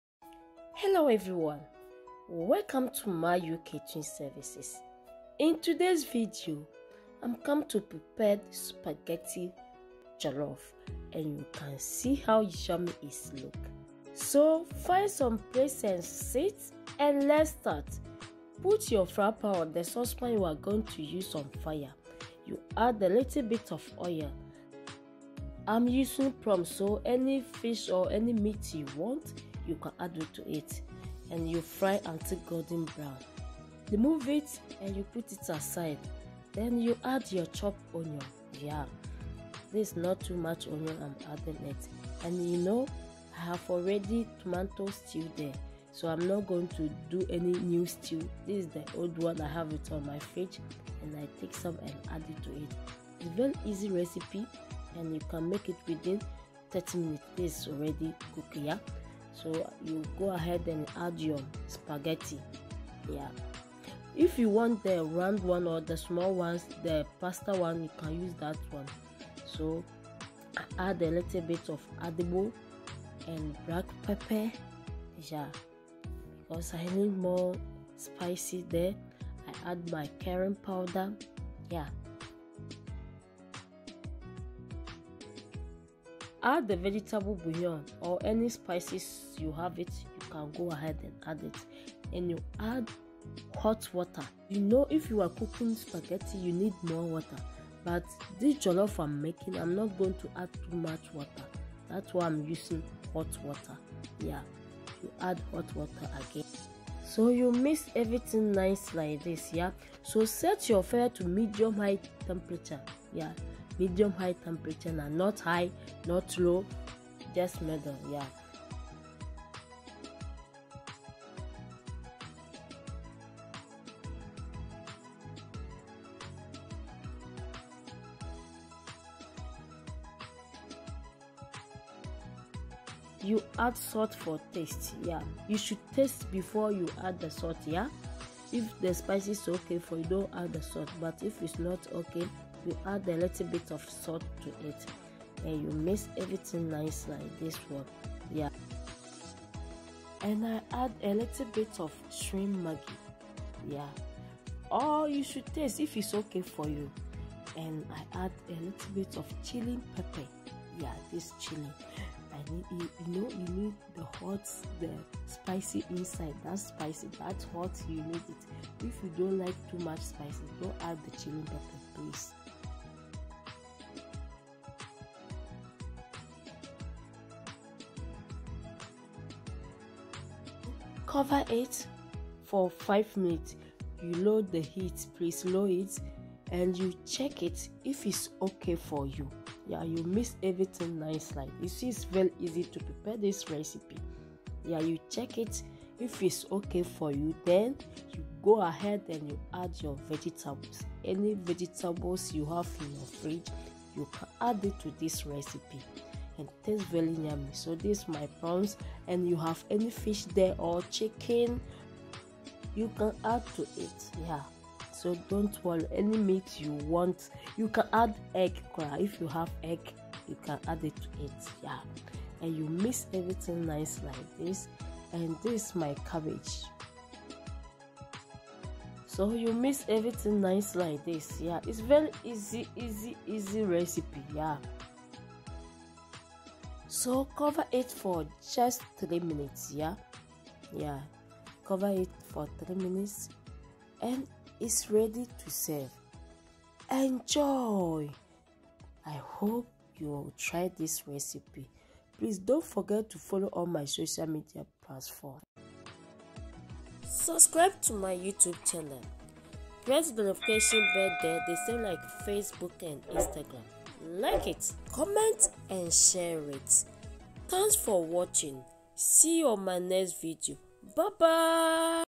How To Make Delicious Spaghetti Jollof.
Hello Everyone 👋,

INGREDIENTS
Seasoning The Prawns / Shrimp with
Fish Seasoning, Adobo, Curry Powder, And Black Pepper
1tbsp Cooking Oil
Onion (cut)
Tomorrow Stew
500g Spaghetti
Seasoning With
 Black pepper
Adobo
Curry Powder
Vegetables Bouillon
Salt For Taste
Shrimp Maggi Powder
Chili powder (Optional)
Water to cook.
Add Any Vegetables is your choice.